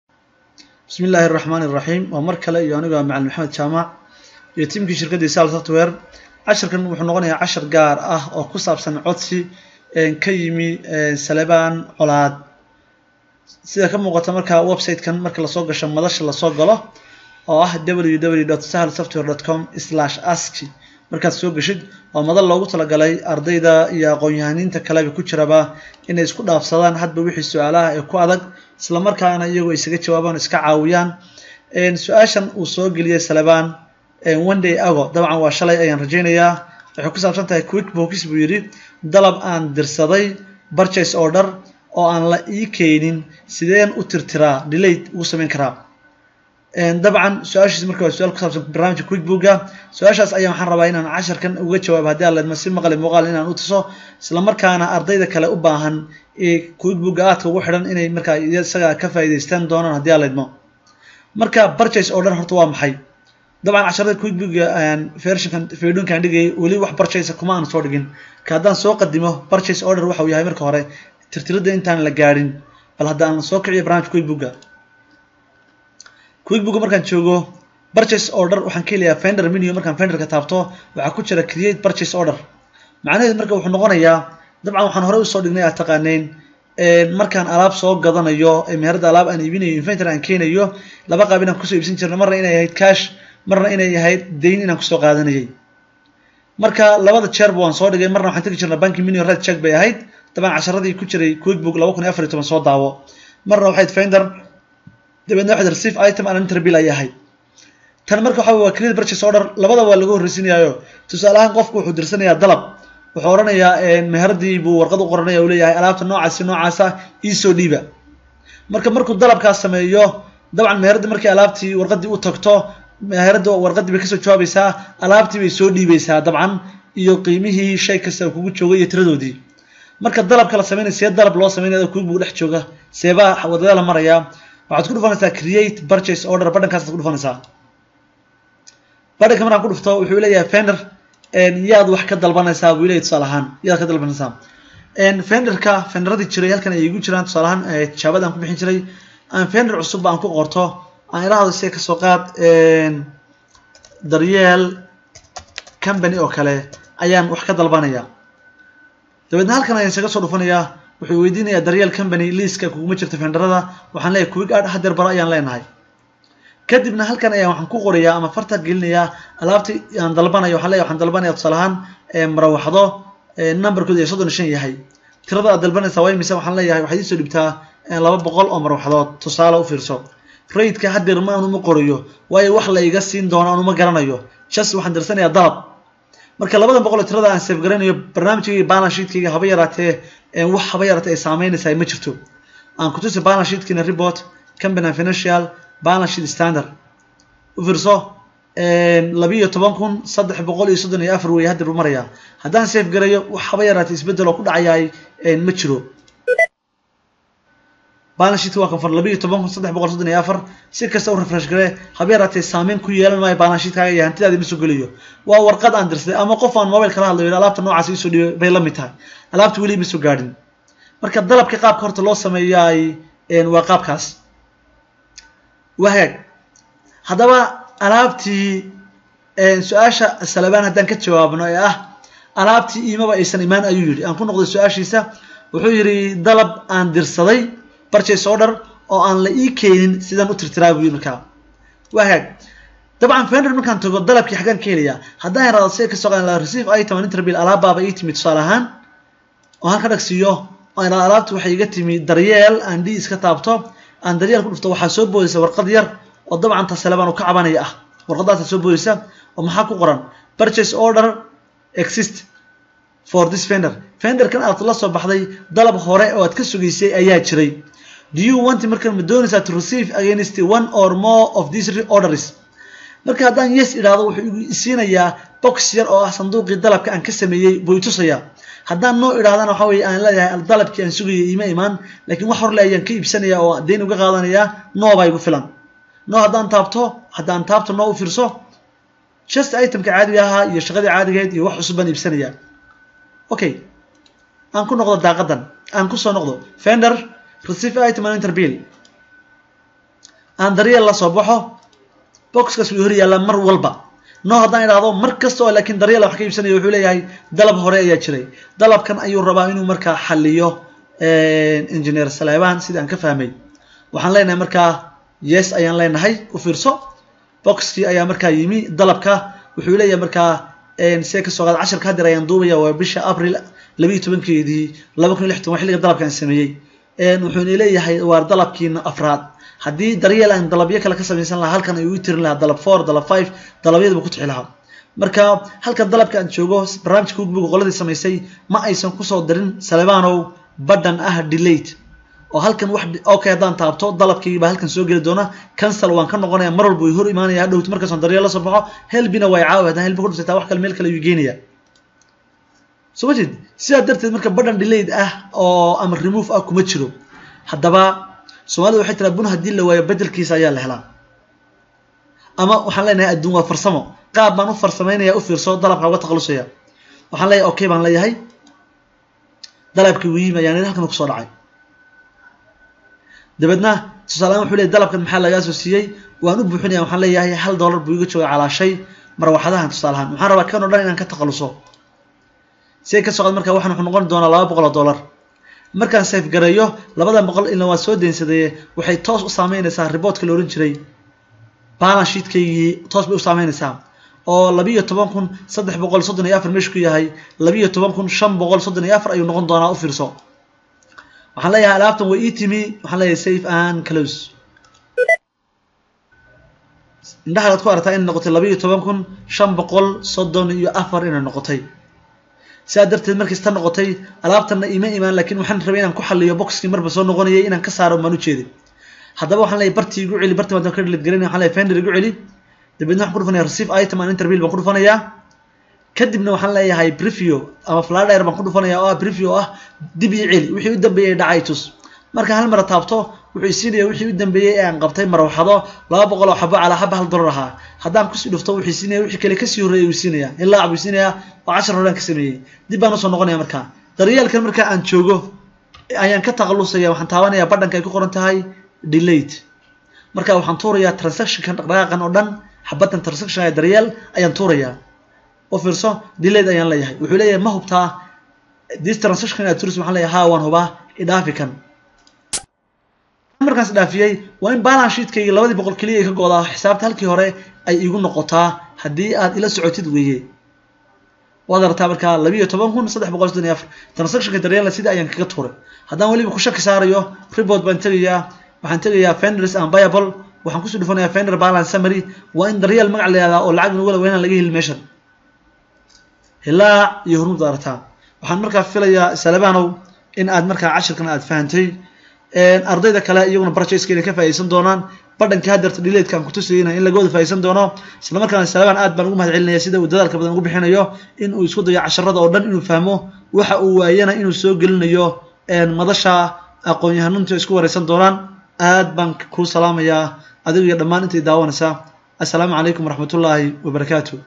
Sidee dalab u sameyn karaa una tirtiri karaa Quickbooks Sahalsoftware
ama cinwaanada sare waxaad ka heleysaa casharo kale oo badan, fadlan gaadhsii asxaabtaada, adiguna hoos ku soo qor sida aad u aragtid